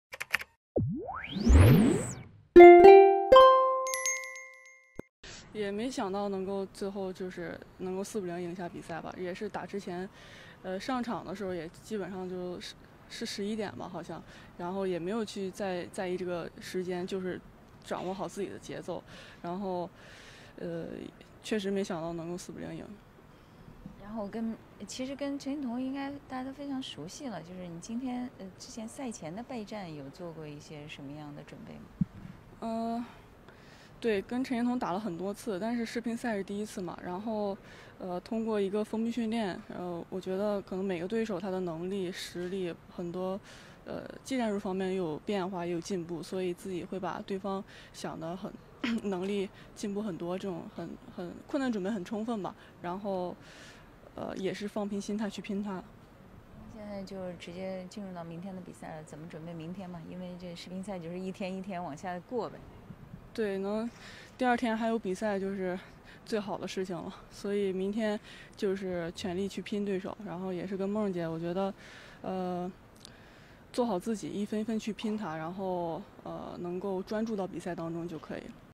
【2021休斯顿世乒赛】王曼昱成功晋级四强赛后采访：没想到会4比0赢下比赛
王曼昱成功晋级四强赛后采访：没想到会4比0赢下比赛